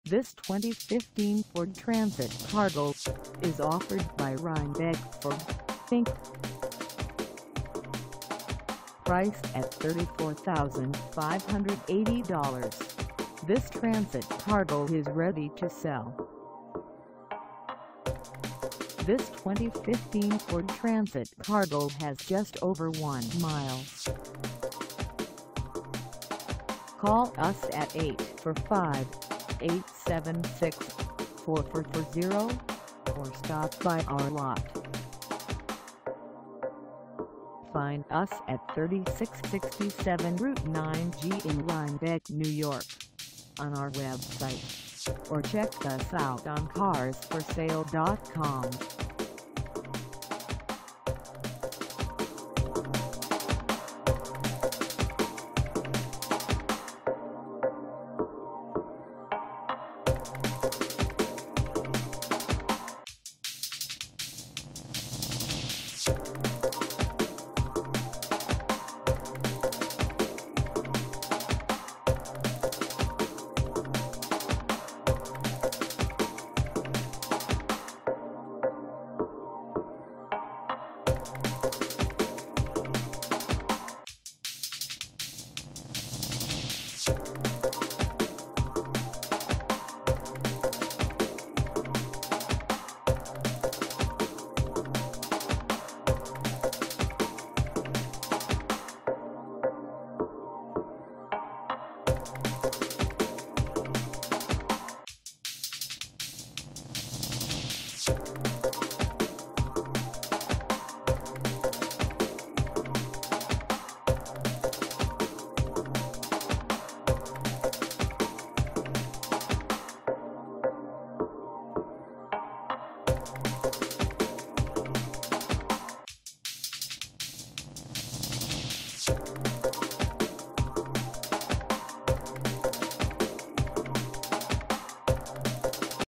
2015 Ford Transit Cargo 250 3dr LWB Medium Roof Cargo Van w/
This 2015 Ford Transit Cargo 250 3dr LWB Medium Roof Cargo Van w/Sliding Passen is for sale in Rhinebeck, NY 12572 at RHINEBECK FORD, INC..
Engine: V6 3.7L V6
Transmission: Automatic 6-Speed
Color: Oxford White
Mileage: 1
Contact RHINEBECK FORD, INC. 3667 Route 9G Rhinebeck, NY 12572 to test drive this 2015 Ford Transit Cargo 250 3dr LWB Medium Roof Cargo Van w/Sliding Passen today.